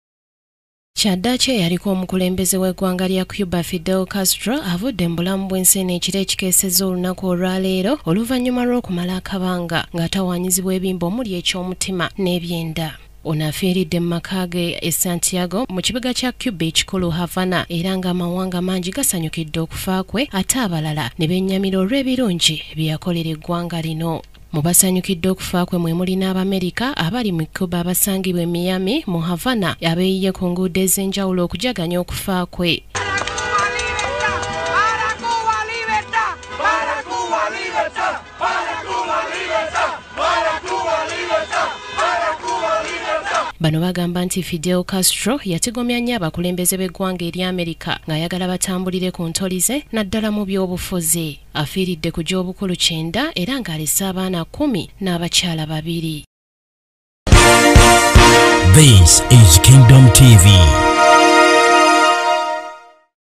Fidel Castro afudde Abamerika bayisizza ebivulu
Abakulembeze b’amawanga  ag’enjawulo  boogedde ku kufa kw’eyaliko omukulembeze w’eggwanga lya Cuba Fidel Alexandro Castrol Ruz ng’abwadde omuntu ow’enjawulo.
Mu bantu abamwogeddeko mwemuli president wa Russia Vladmir Putin amwogeddeko ng’omukulembeze ow’ekyasa,Ssaabaminisita wa Buyindi Nerandra Modi ategezeeza nga bw’abadde ow’eky’okulabirako mu kyasa ky’abiri ng’ate ye omukulembeze wa Bufalansa Francoise Hollande naye amwogeddeko ng’omusajja abadde omumalirivu.
Fidel Castrol afudde mu kiro ekikeeseza olunaku olwaleero era nga gavumenti erangiridde ennaku mwenda ez’okukungubaga. Wadde ng’abamu bamwogeddeko ebirungi,bo Bannansi ba Amerika basiibye bagyaganya olw’okufa kwe.